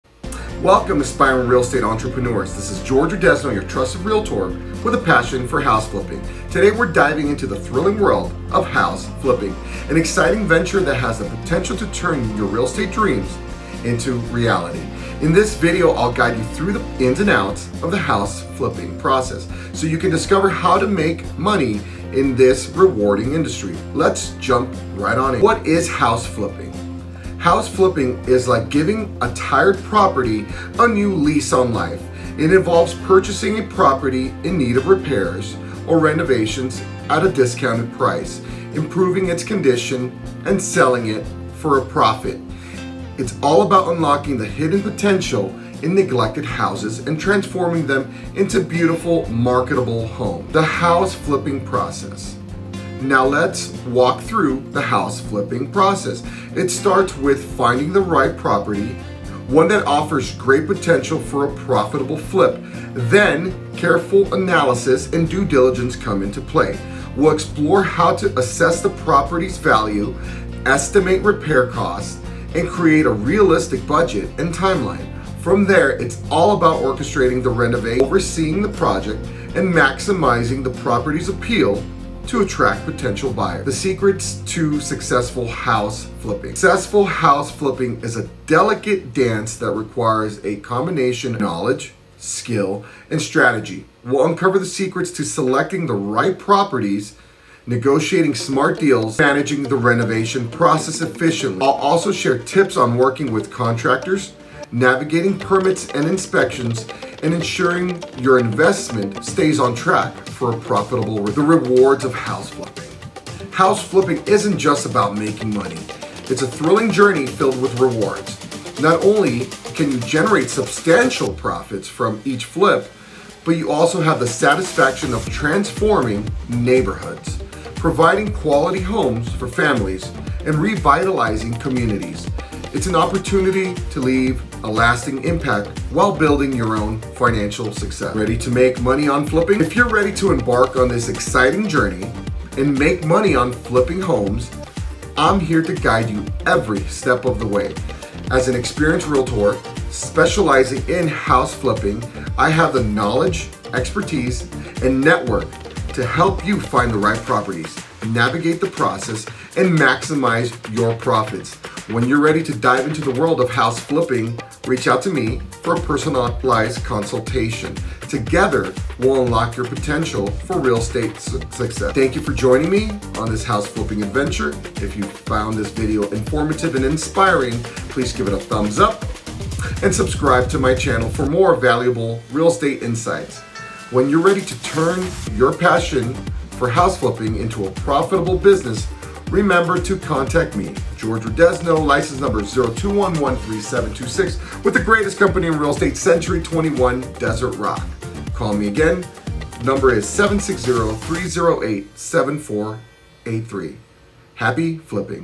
Flippin’ Easy |House Flipping 101| Jorge A Rodezno Realtor
Welcome to the exhilarating world of house flipping! Join Realtor Jorge Rodezno in this engaging video as he reveals the secrets to house flipping success. Whether you're a seasoned investor or just starting out, this video is your ultimate guide to making money in the thrilling realm of real estate flipping. Jorge shares valuable insights on finding the right properties, estimating repair costs, managing renovations, and maximizing profits. Get ready to unlock your potential and turn neglected houses into stunning homes that sell like hotcakes. Ready to flip your way to financial freedom? Reach out to Jorge Rodezno, your trusted Realtor, for expert guidance and personalized advice. It's time to dive into the world of house flipping and turn your passion into profit. Let's get flippin'!"